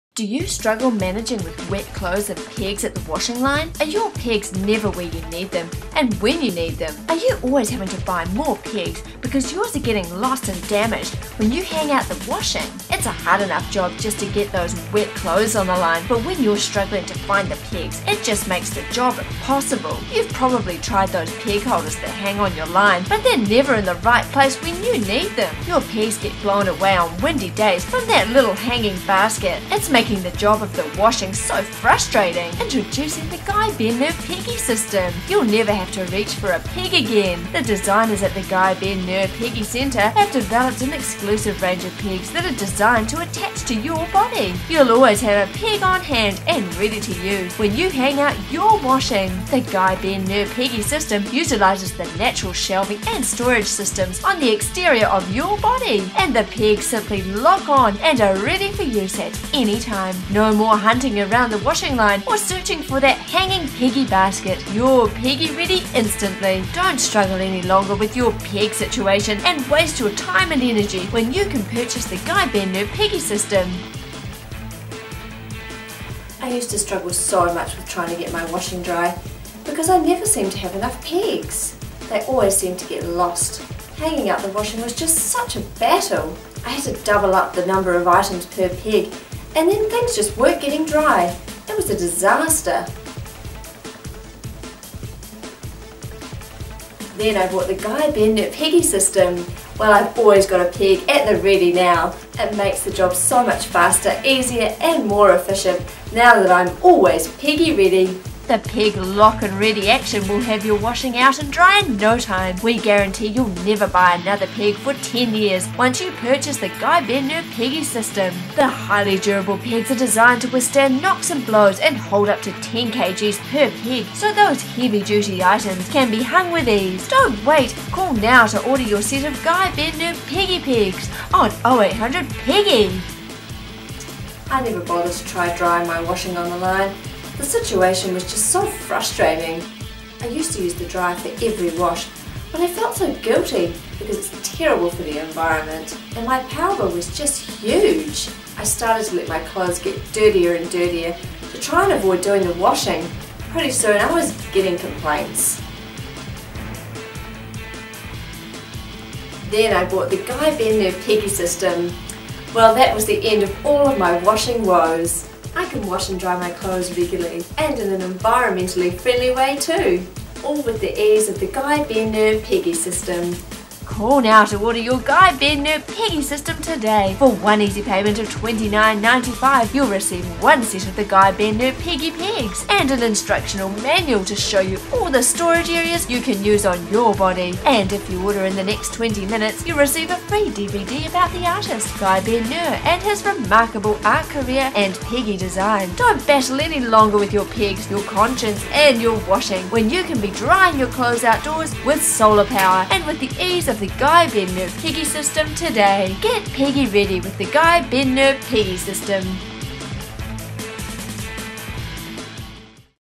Guy Ben Ner Peggy System
"Guy Ben Ner Peggy System" was inspired by a Guy Ben Ner video - watch from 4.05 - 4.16.

In 2014 I produced 50 videos using 50 artists to complete 50 household chores in 100 days. The initial project transformed artists work into techniques for domestic tasks, now these domestic techniques will be commercialized, supported by an infomercial for each artist.